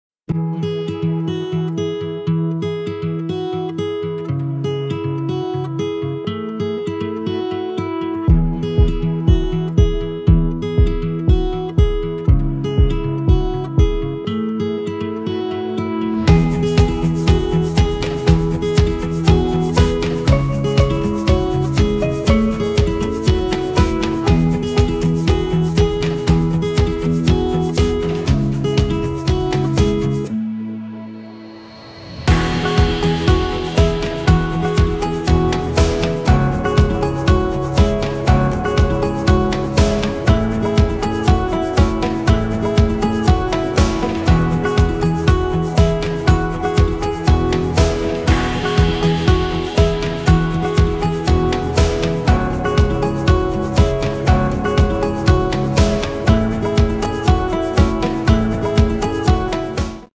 Real Estate Commercial Promo Video - Hassan Farrow
Developer Devin Groody has created this service real estate  promotional video for new luxury homes in Greenwich, Connecticut.
Real Service Luxury Real Estate Video